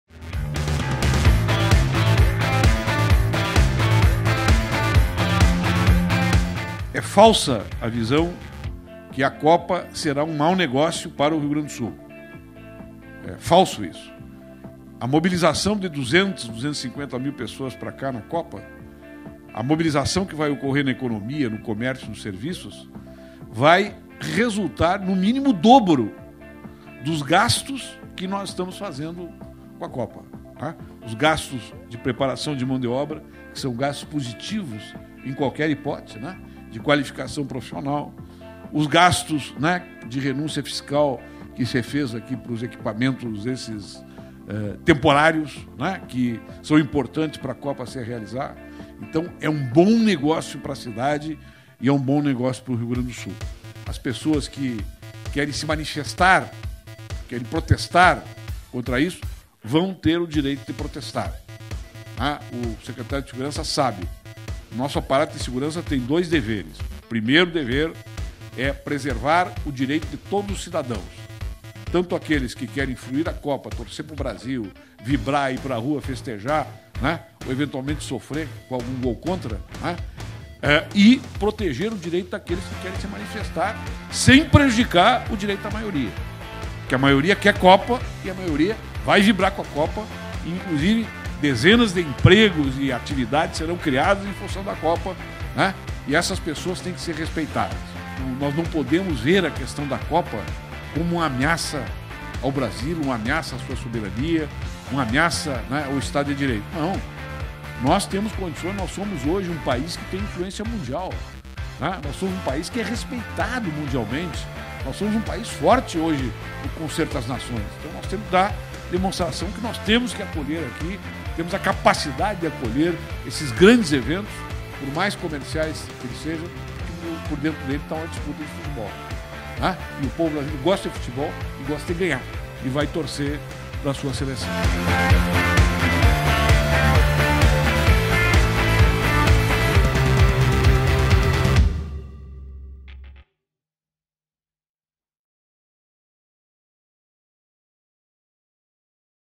Governador Tarso Genro fala sobre a Copa do Mundo
Não é de hoje que o Gabinete Digital debate com a sociedade o tema da Copa do Mundo no Brasil. Desde 2012 o Governo do Estado do RS tem procurado dialogar entre vários eixos da sociedade para construirmos um evento mais democrático. O Governo Responde: Diálogos da Copa, realizado no dia 9 de junho, reuniu o governador Tarso Genro, secretários de governo e diversas representações da sociedade civil fará um último debate sobre o tema.

Confira o que o governador Tarso Genro disse sobre o evento que acontece na capital dos gaúchos.